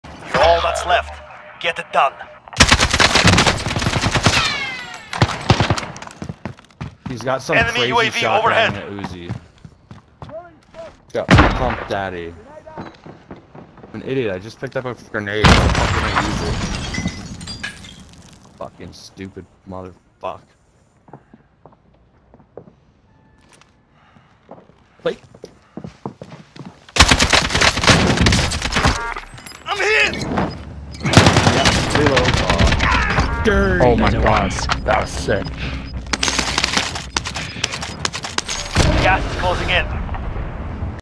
Molotov out
Molly kill/team wipe. 2 shots left in the mag and no health.